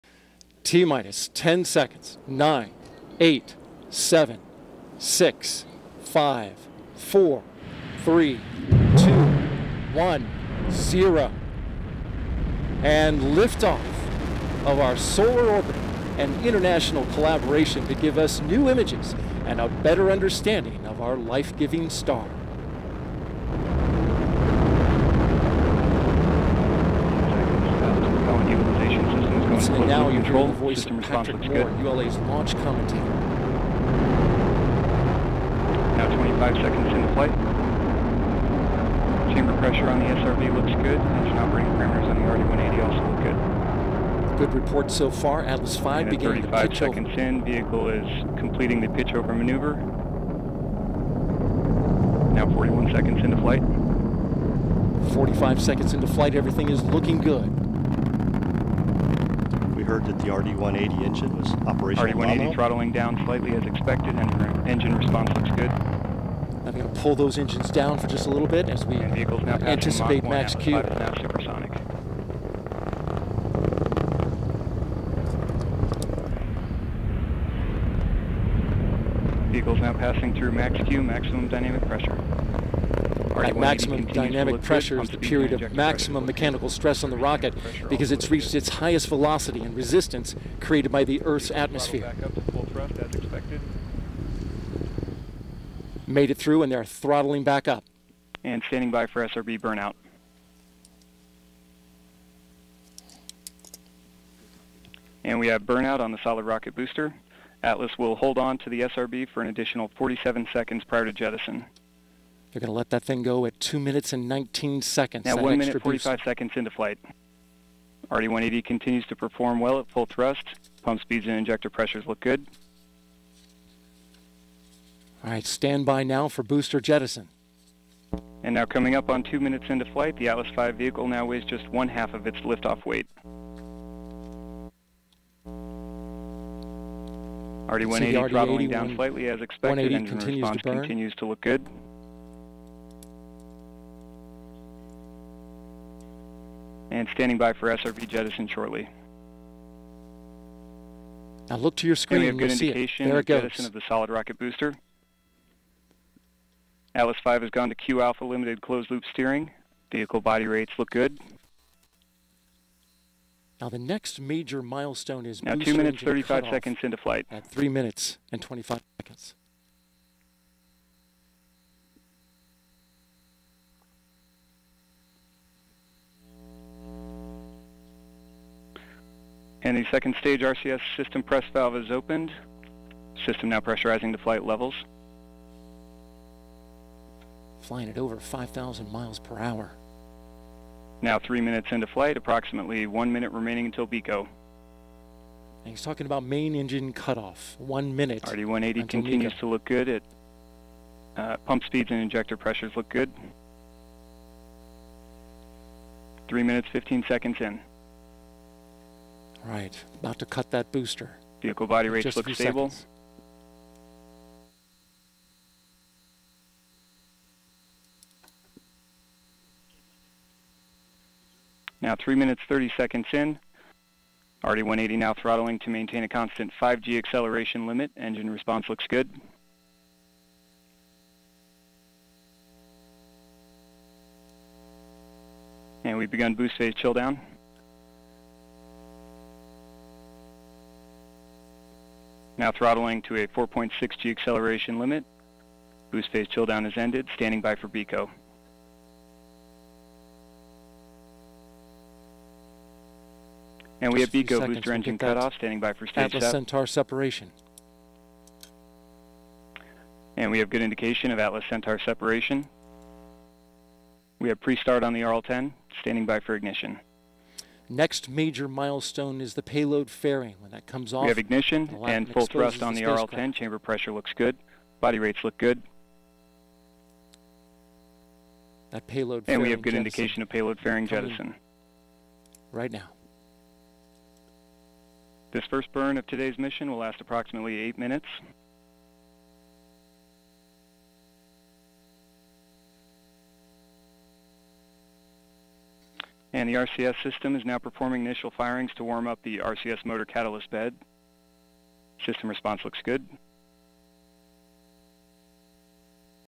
Blastoff! New Solar Orbiter launches atop Atlas V rocket
The ESA/NASA Solar Orbiter launched atop a United Launch Alliance Atlas V rocket on Feb. 9, 2020 from the Cape Canaveral Air Force Station in Florida. Story: Solar Orbiter launches on historic mission to study the sun's poles

Credit: NASA